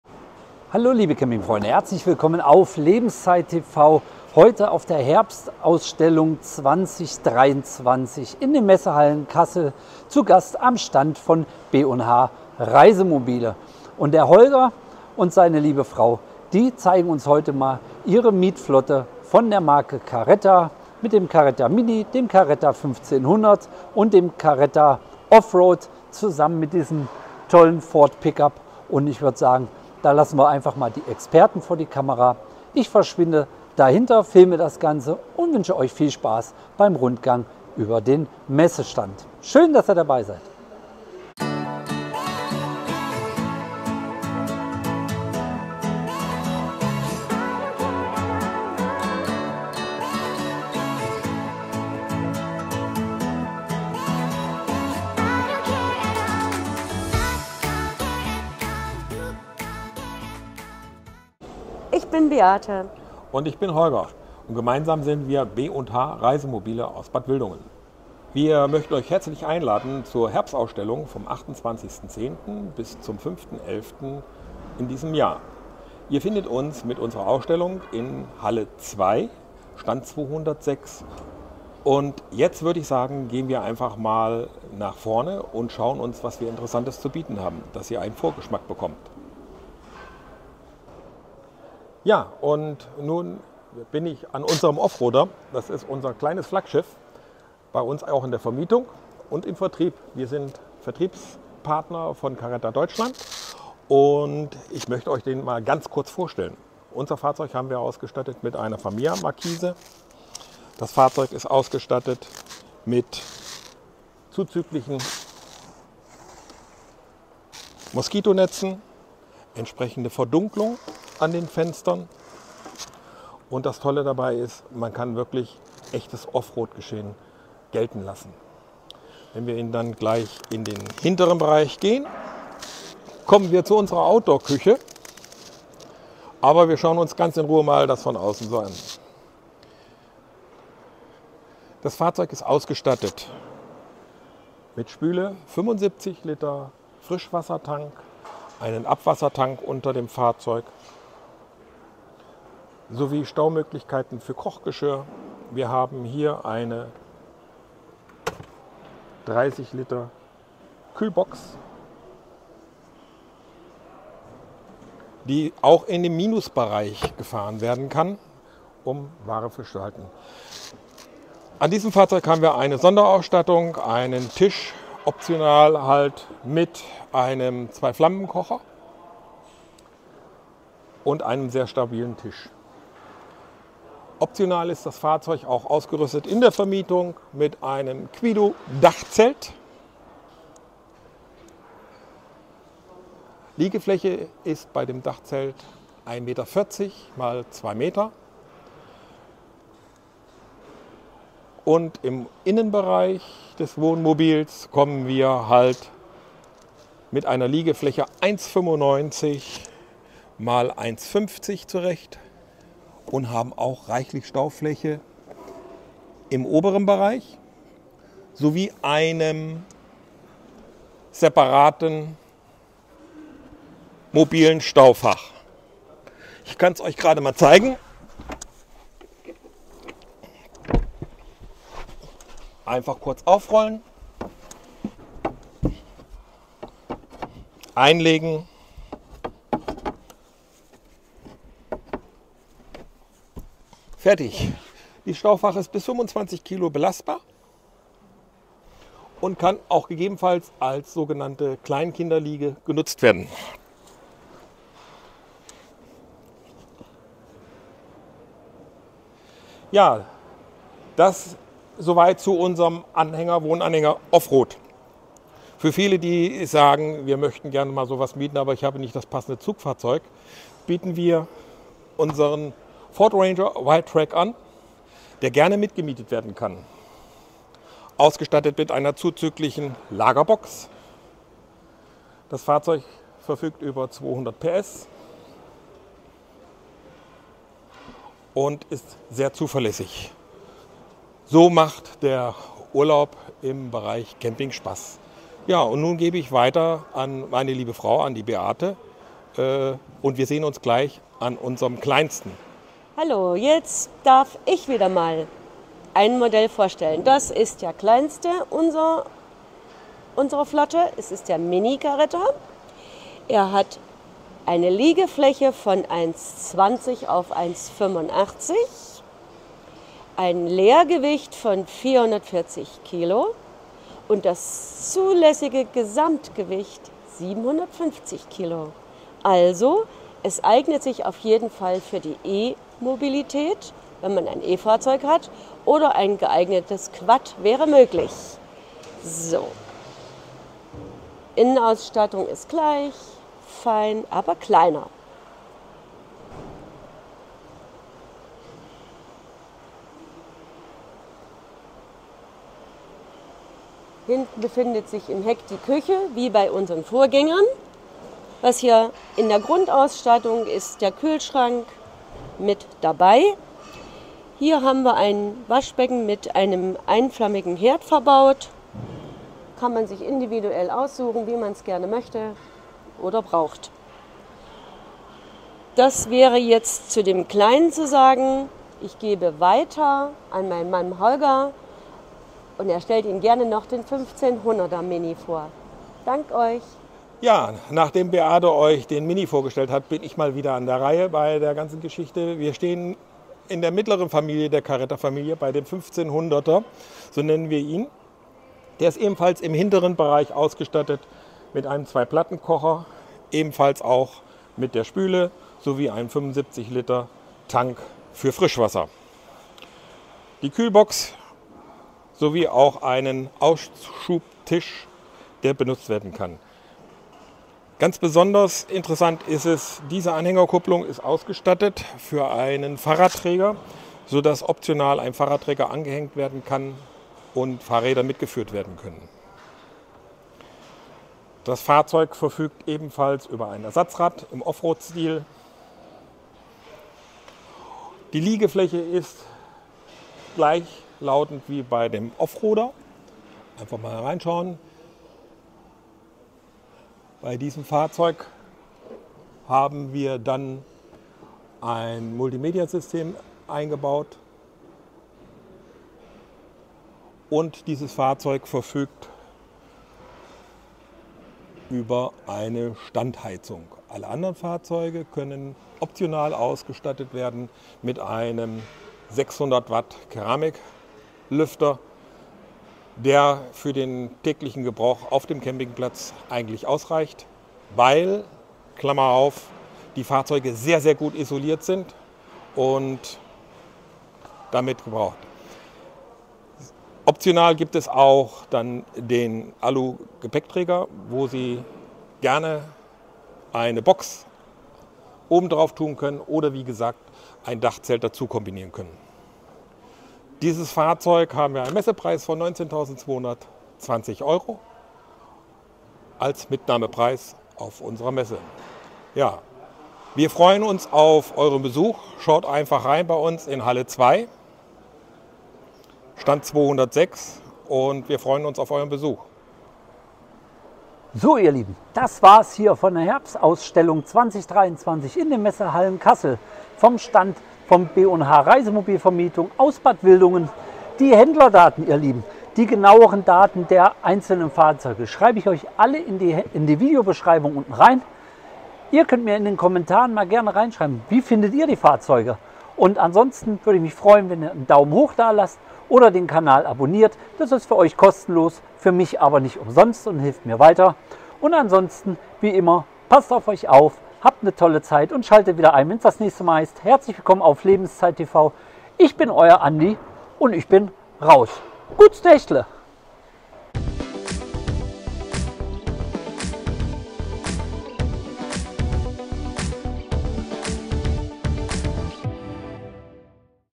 Caretta Mini, Caretta 1500 & Caretta OffRoad
B & H Reisemobilvermietung
Vertrieb für Caretta Deutschland

St.-Florian-Straße 3a
34537 Bad Wildungen

Caretta Mini

LxBxH in mm
4000x1760x1540

Liegefläche BxL  in mm
1200x1899

45 l Frischwassertank
35 l Kompressorkühlbox

Leergewicht: 390 Kg
Zul. Ges.Gewicht: 750 Kg
Stützlast: 70 Kg

100 km/h Zulassung

Caretta 1500

LxBxH in mm
4250x2150x1635

1509x1900

45 l Frischwassertank
35 l Kompressorkühlbox

Leergewicht: 510 Kg
Stützlast: 70 Kg

100 km/h Zulassung

Caretta OffRoad

LxBxH in mm
4500x2150x1840

1500x1900

45 l Frischwassertank
35 l Kompressorkühlbox

Leergewicht: 620 Kg
Zul.Ges.Gewicht: 1000 Kg
Stützlast: 90 Kg

100 km/h Zulassung